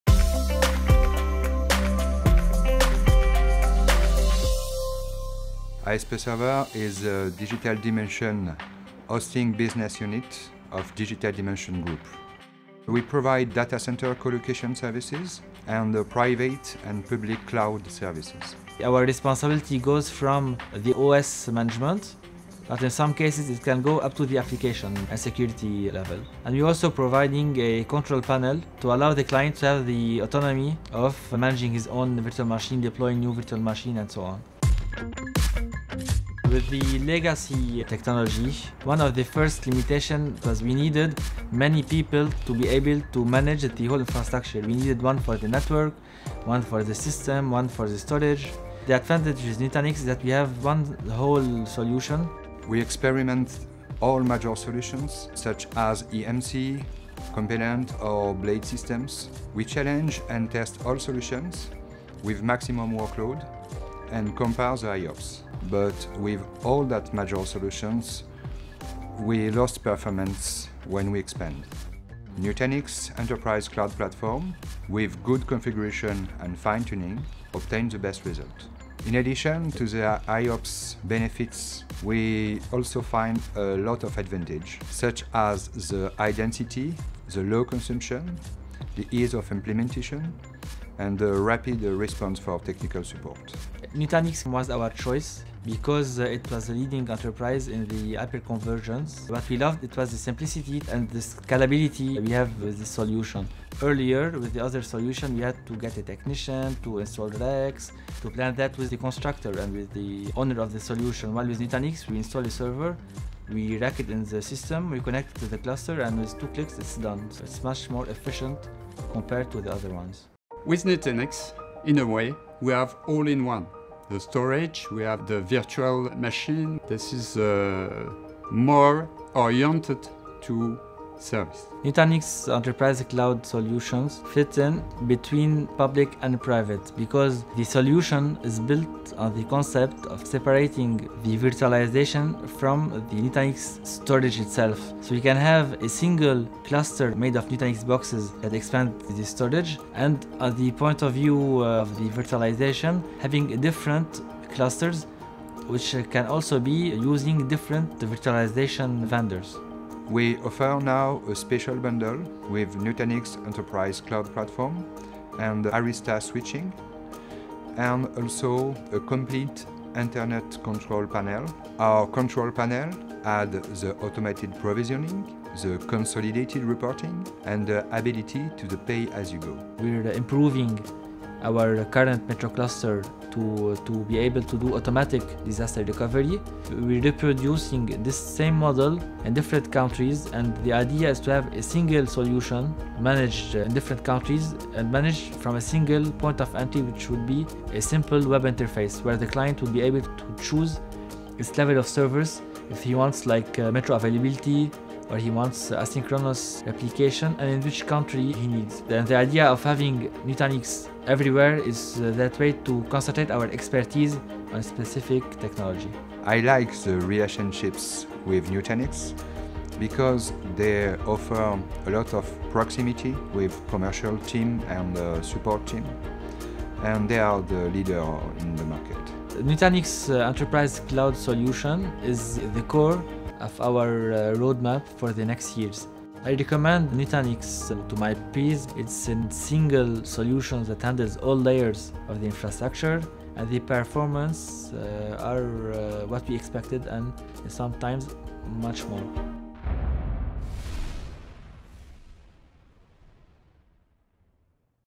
ASP Serveur Run Cloud Services on the Nutanix Enterprise Cloud Platform
Hear how ASP Serveur, the leading French enterprise-class service provider, has simplified operations by running services on the Nutanix Enterprise Cloud Platform